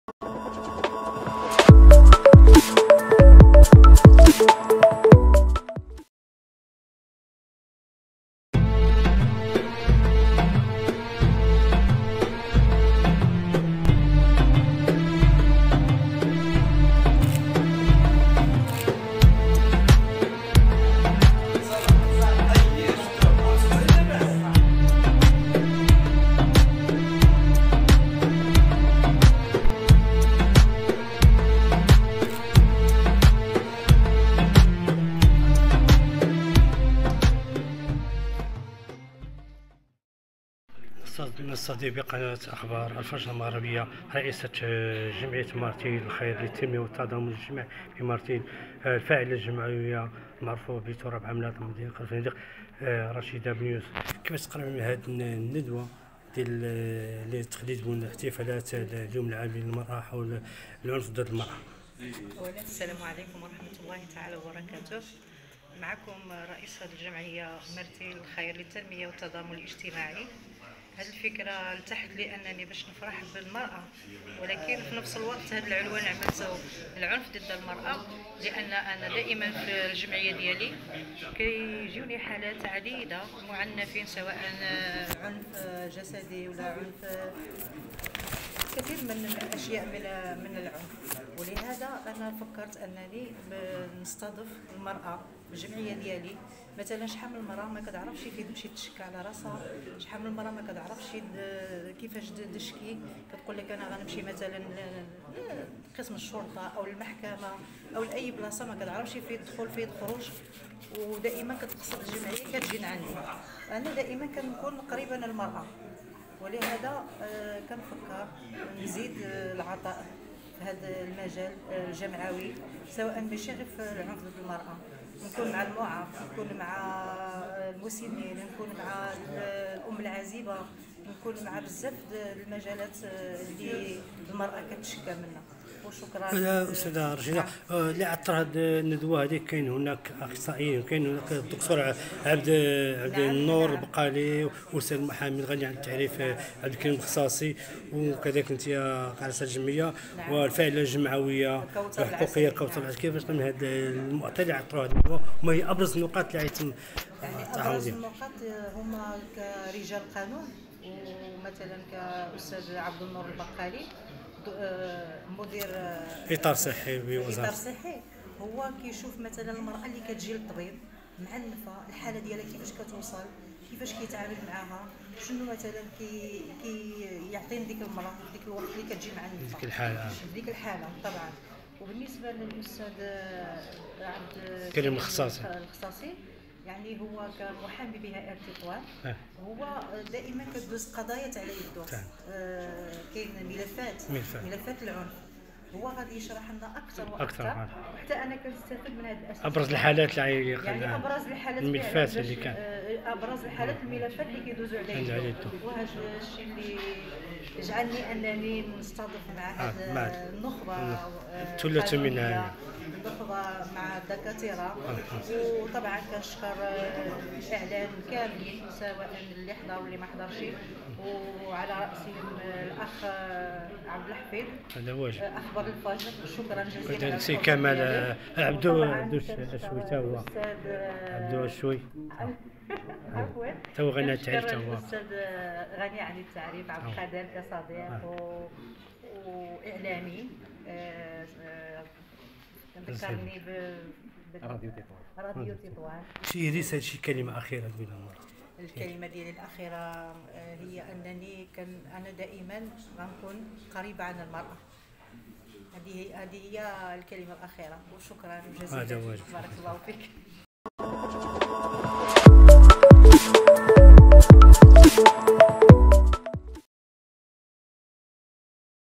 تصريح مع رشيدة بن يوسف رئيسة جمعية مرتيل الخير : استقبل عديد من الحالات العنف ضد المرأة
تصريح مع رشيدة بن يوسف رئيسة جمعية مرتيل الخير : استقبل عديد من الحالات العنف ضد المرأة عنف جسدي ، معنوي. .

الفايسبوك :

النسخة الفرنسية

 النسخة العربية

عشاق الرياضة

 التويتر

الأنستغرام

البريد الإلكتروني